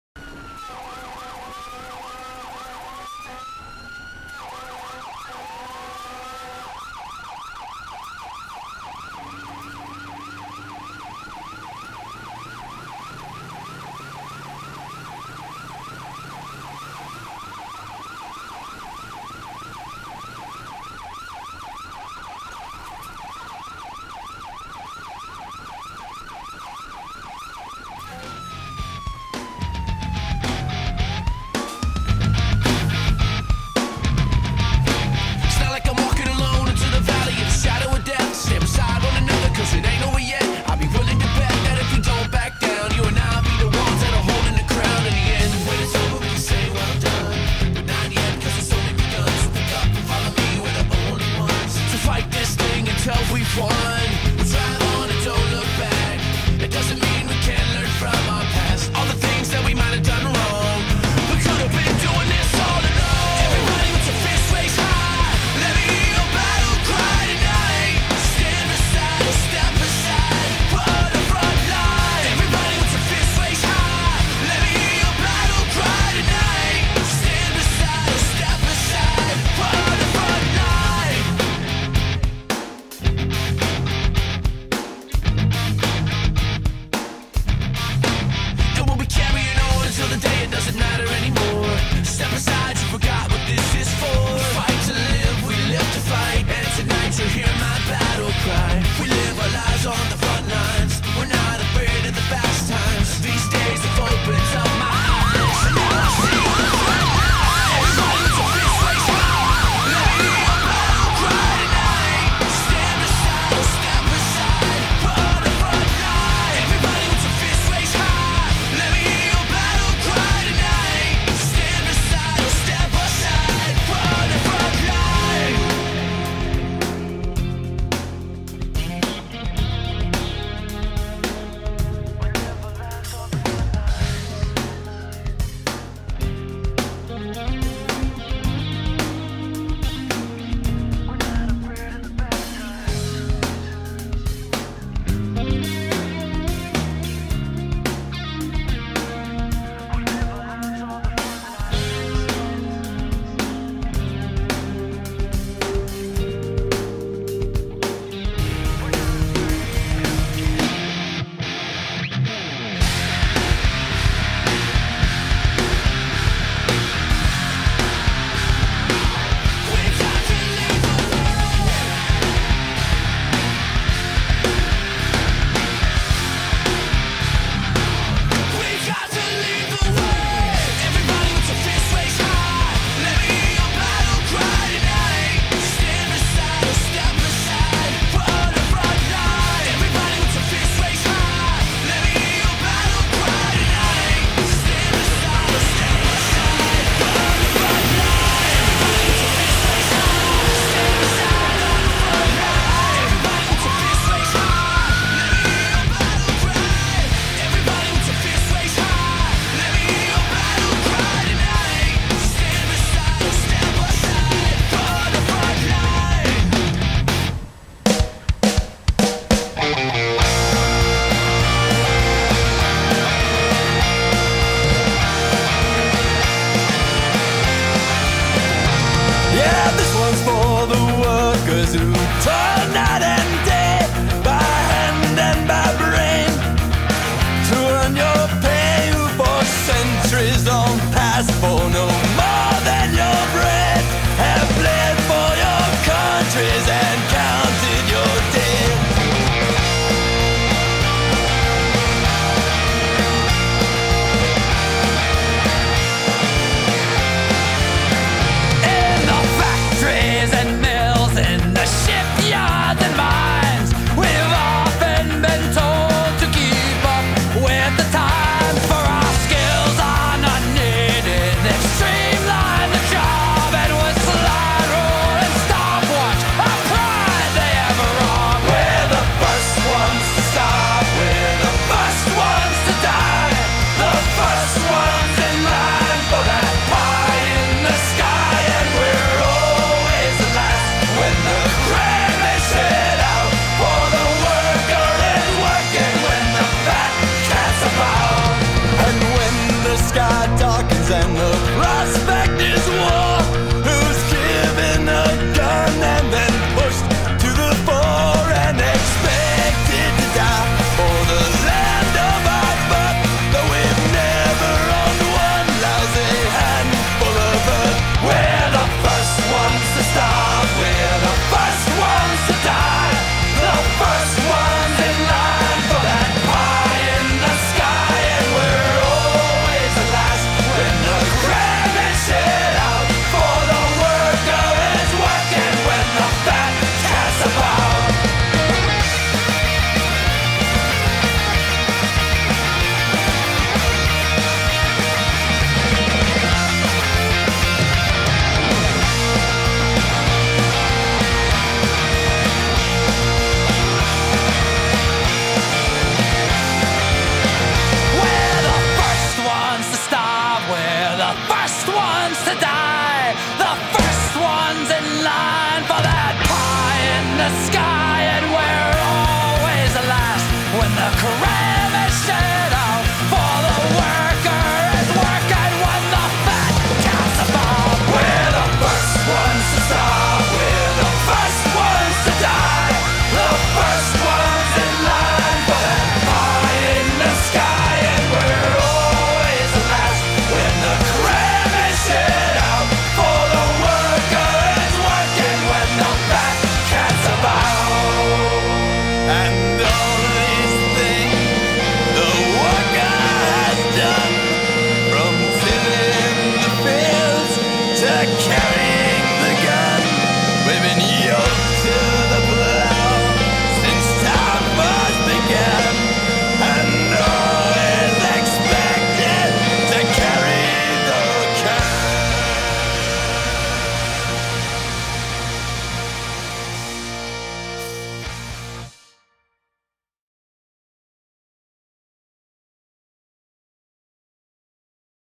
Douglas Fire Department 2012
Pictures and video put together for the Douglas Wyoming Fire Department